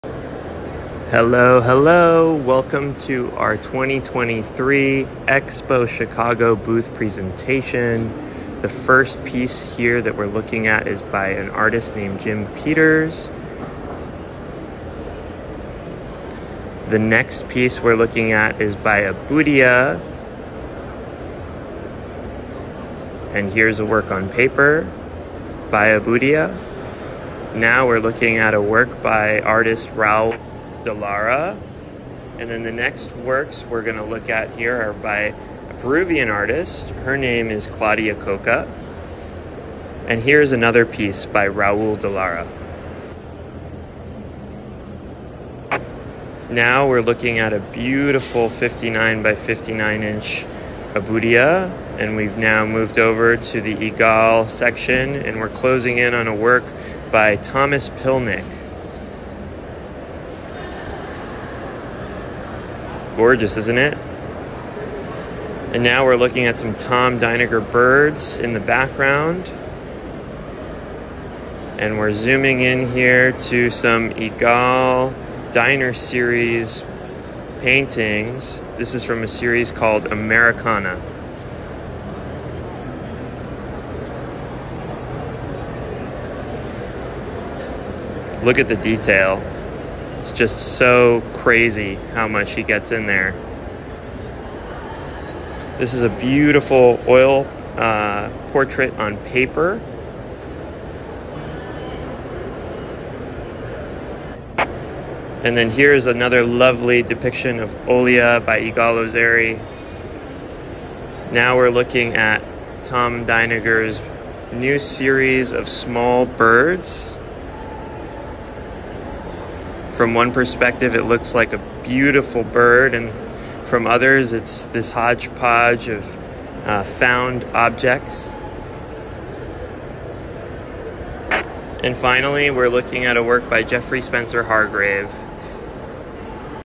EthanCohenGallery Booth Tour 458!
ethancohengallery Happy Thursday ✨

Here’s our booth tour for the EXPO CHICAGO art fair.